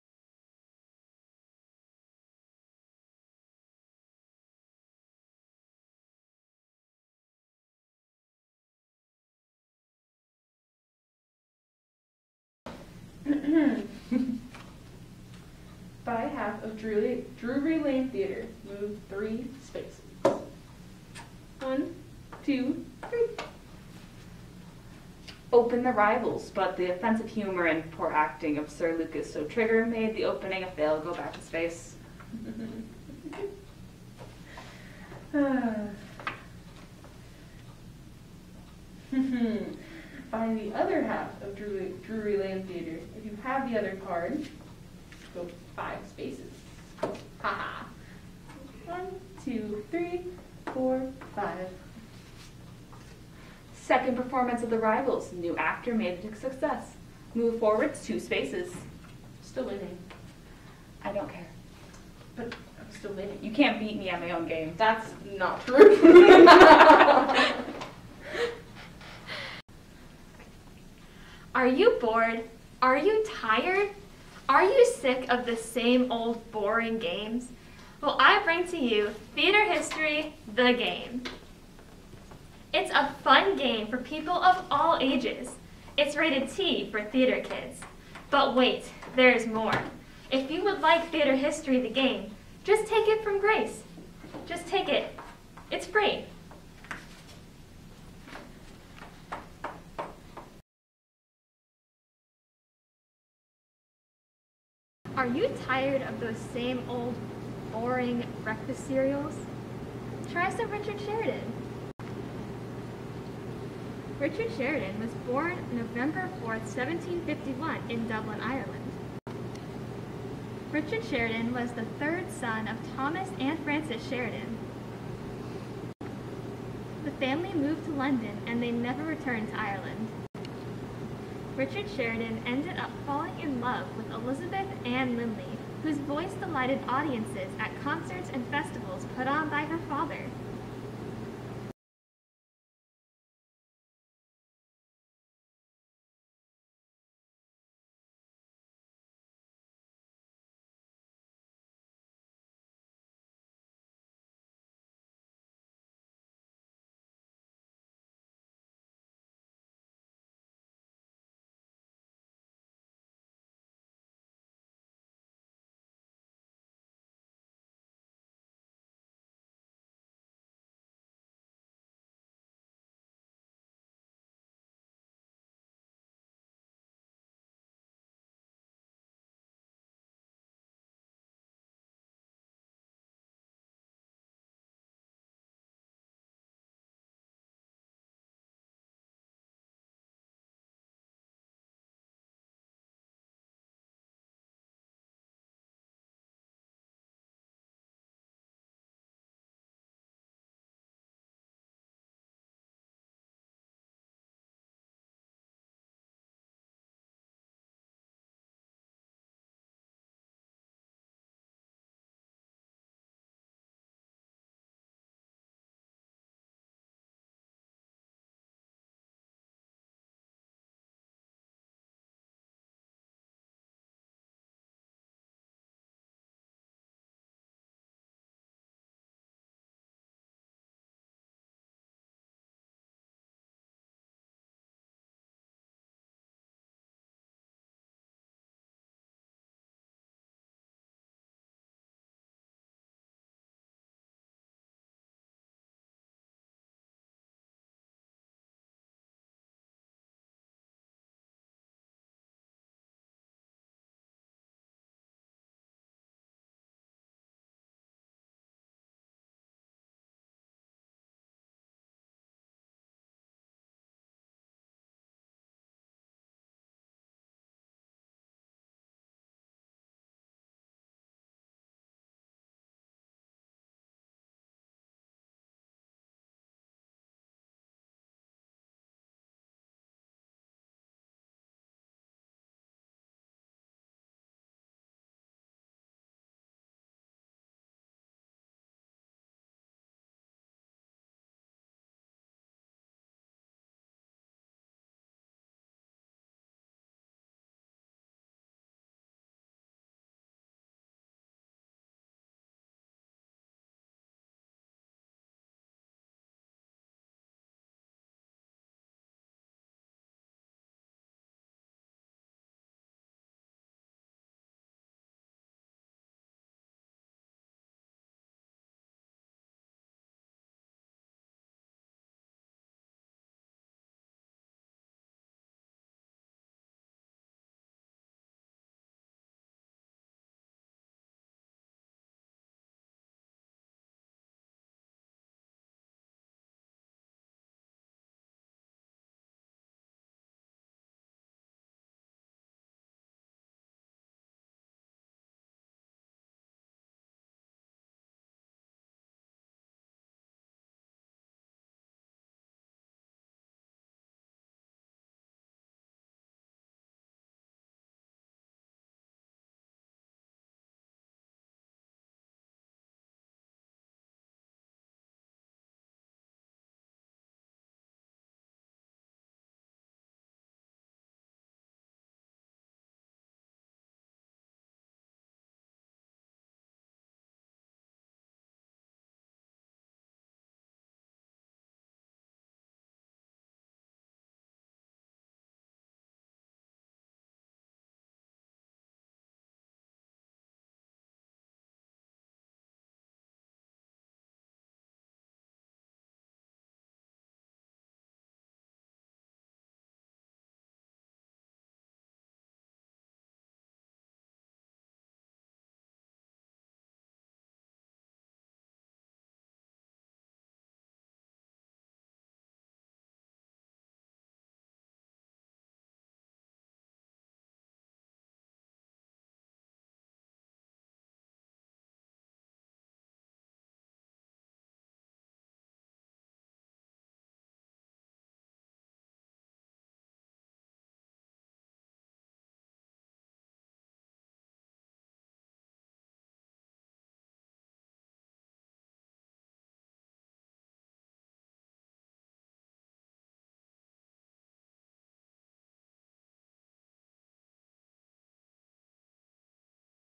Richard Sheridan
this is my theater history project on Richard Sheridan.